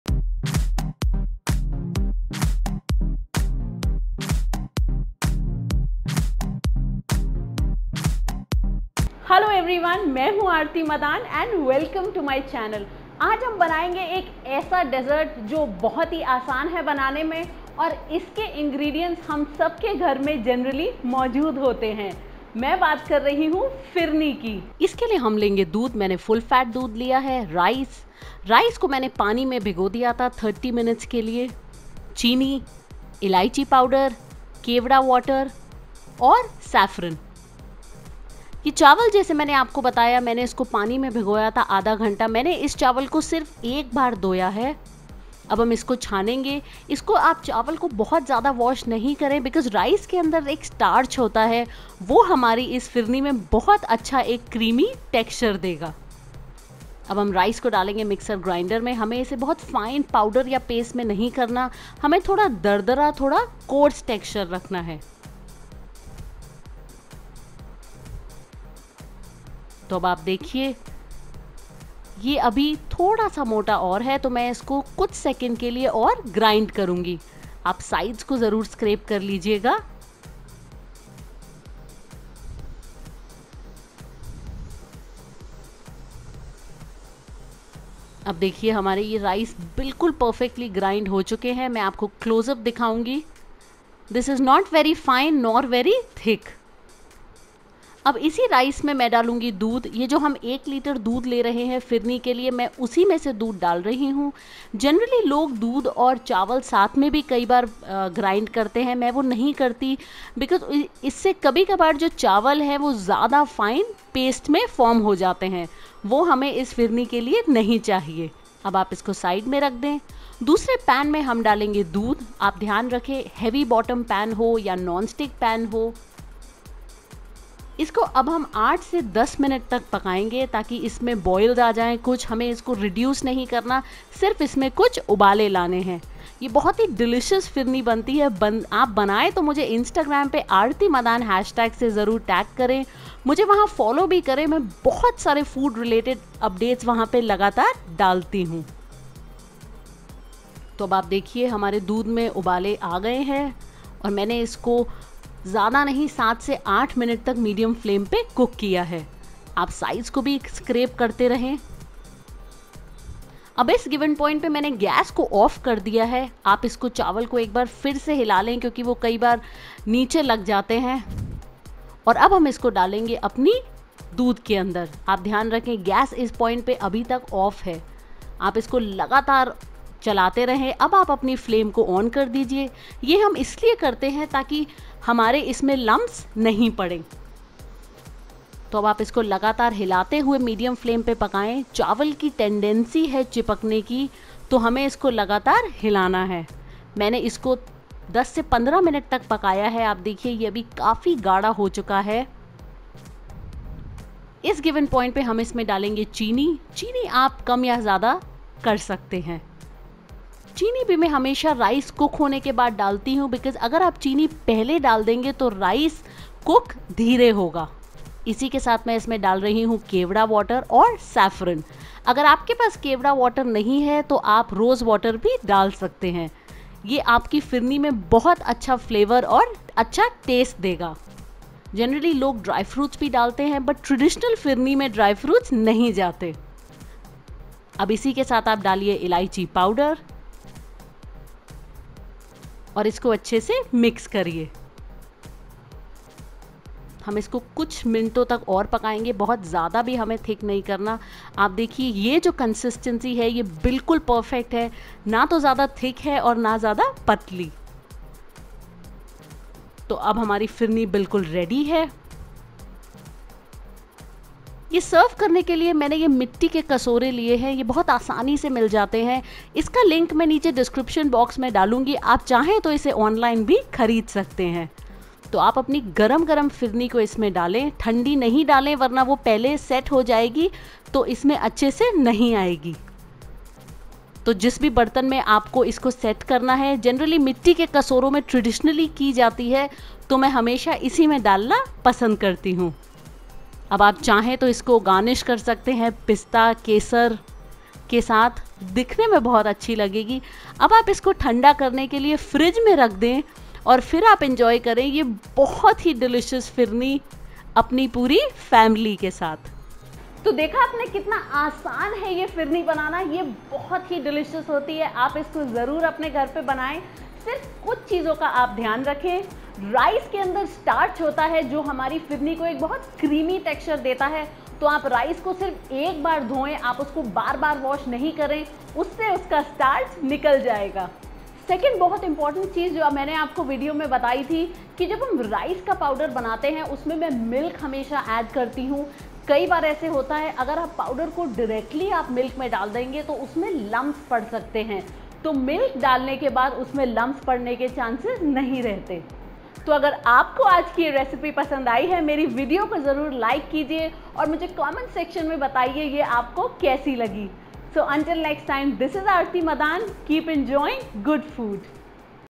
Phirni Recipe | How to Make Firni At Home | Eid Dessert Recipe (Hindi)
Yummy Phirni recipe that is super quick and very easy to make. Also known as Firni by some, this is made with rice and milk as the main ingredients.

Equipment Used:
High-power induction cooktop
Long-lasting non-stick kadhai set
Clay bowls